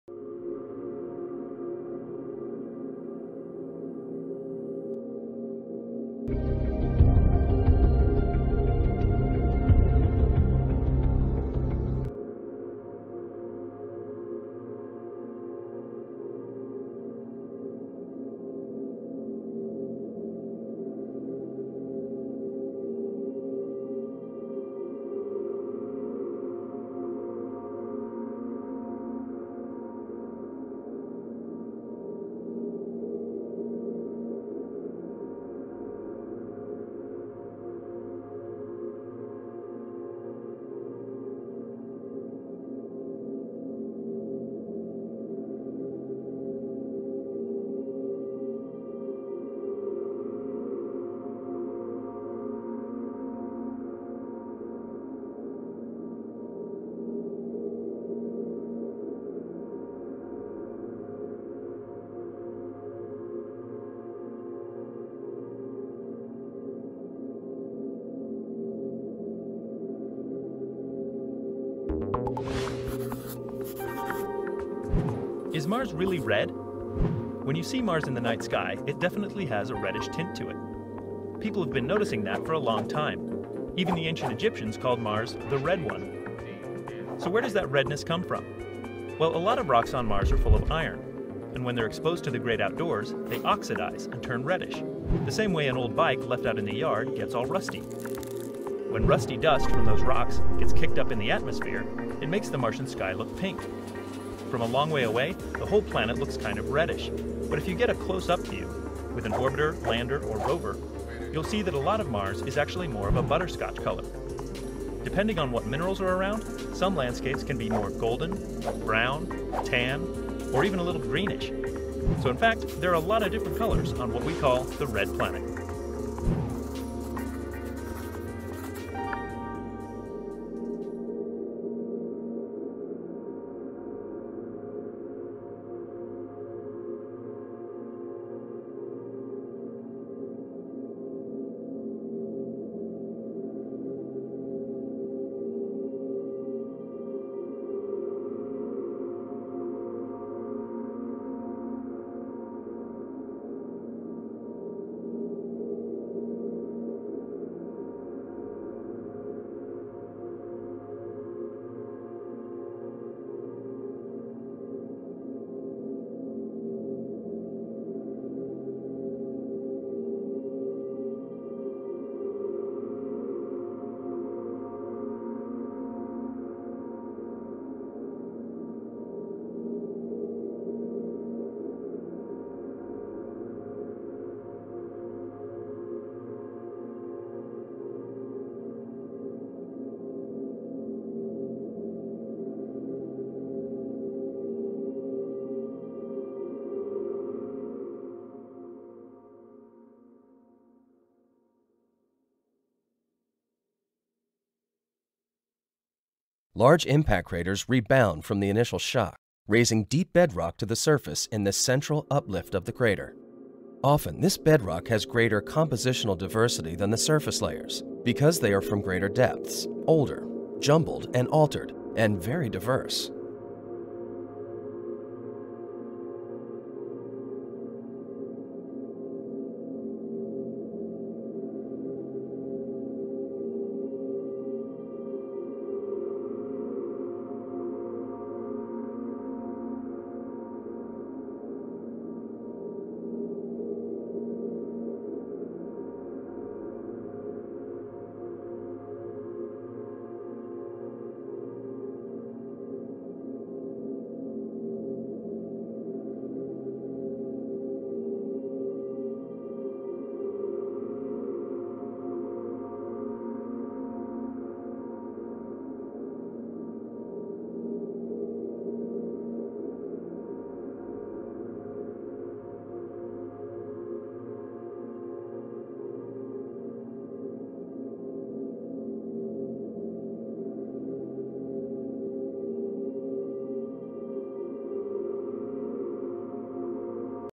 Mars TRUE COLOR, Rover and Satellite Images, Nasa Science: Blue Sky of Mars
NASA explains the true color of Mars, Orange filters on the exploration Rovers revealed at this link:

NASA has pierced the firmament, traversed the van allen belt, and beamed a signal some millions of miles to bring you these pictures of MARS!